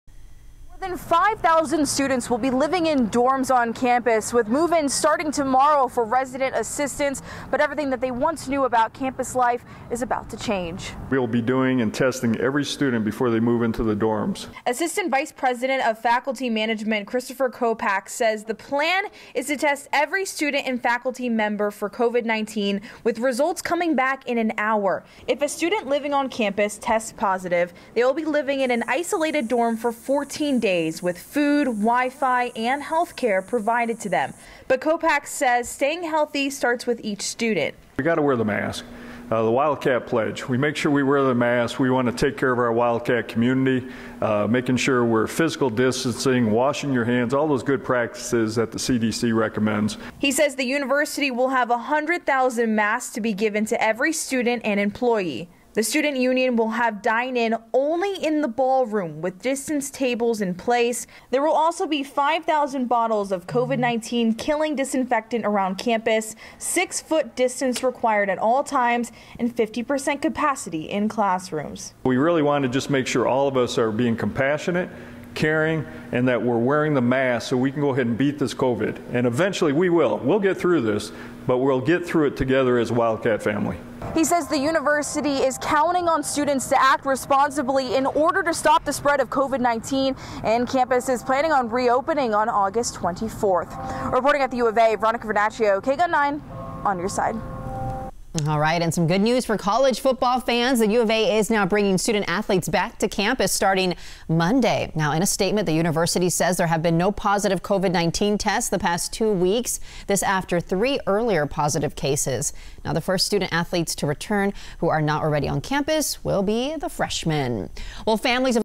UArizona plans to test all students and employees for coronavirus, plans safe return to school
For the past 18 weeks, the University of Arizona has been planning a safe return for students and employees.